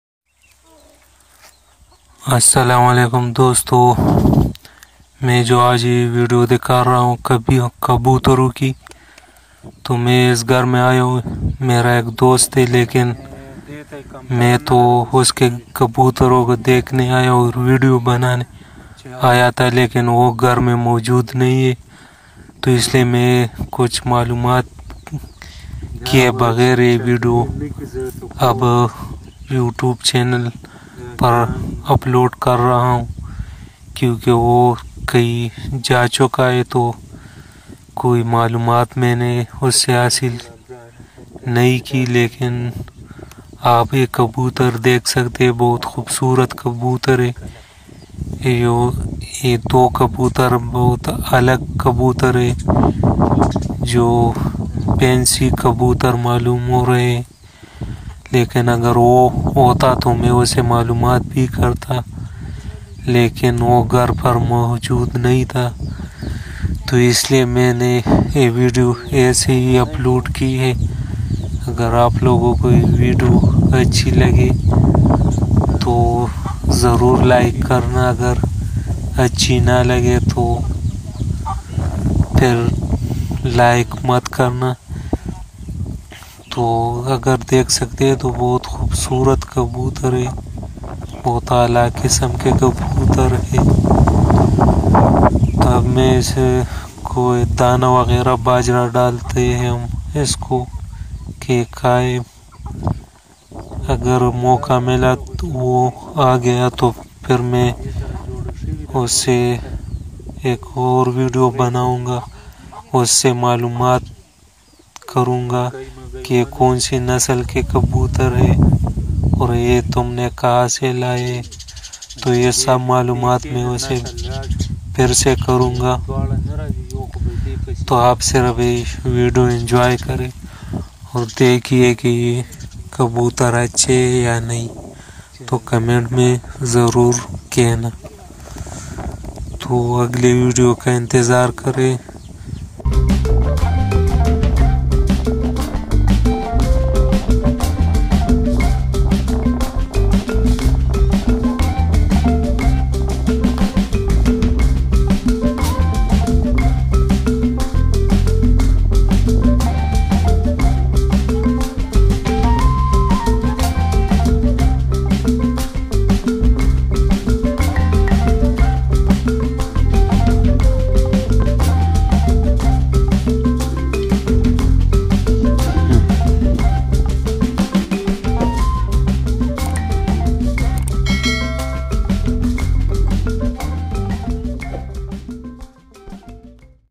Most beautiful pigeons / two fancy pigeon /nbg animal
Friends السلام علیکم my name is Niaz and nbg animal is making another video

Is a beautiful pigeon Hey my friend's house I had to get him some Information But is not at home He has a pigeon It also has two fancy pigeons It's a different kind of pigeon I pour some grains